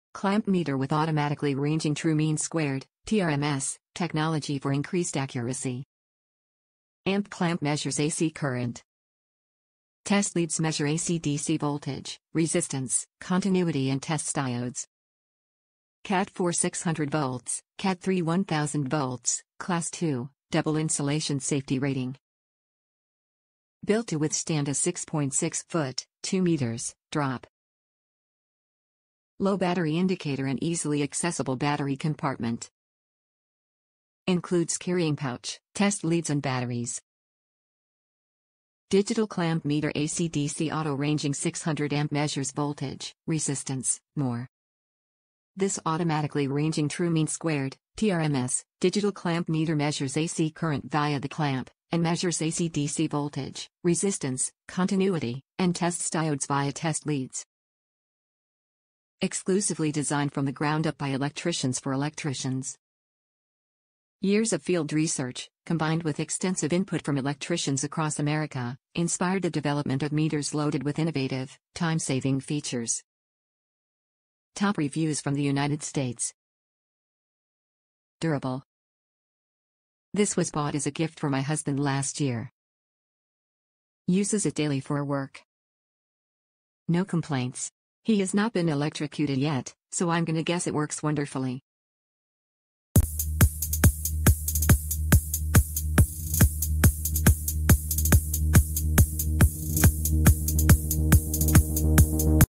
Klein Tools CL600 Electrical Tester, Digital Clamp Meter has Autorange TRMS, Measures AC Current
Clamp Meter with automatically ranging true mean squared (TRMS) technology for increased accuracy
Amp Clamp measures AC current
Test leads measure AC/DC voltage, resistance, continuity and tests diodes
CAT IV 600V, CAT III 1000V, Class 2, Double insulation safety rating
Built to withstand a 6.6-foot (2 m) drop
Low battery indicator and easily accessible battery compartment
Includes carrying pouch, test leads and batteries
Digital Clamp Meter AC/DC Auto-Ranging 600 Amp Measures Voltage, Resistance, More
This automatically ranging true mean squared (TRMS) digital clamp-meter measures AC current via the clamp, and measures AC/DC voltage, resistance, continuity, and tests diodes via test-leads. Exclusively designed from the ground up by electricians for electricians. Years of field research, combined with extensive input from electricians across America, inspired the development of meters loaded with innovative, time-saving features.

Durable
This was bought as a gift for my husband last year. Uses it daily for work. No complaints! He has not been electrocuted yet, so I’m gonna guess it works wonderfully